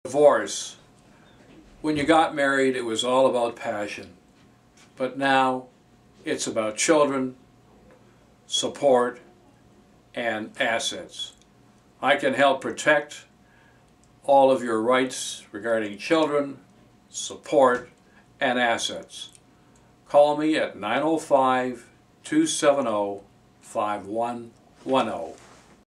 The Passion of Divorce; Stanley Gelman
Stan Gelman reminds us that we married for passion and now we must be mindful of custody, support and assets issues when divorcing in Toronto Mississauga or Oakville.   Have an experienced lawyer help you Stan Gelman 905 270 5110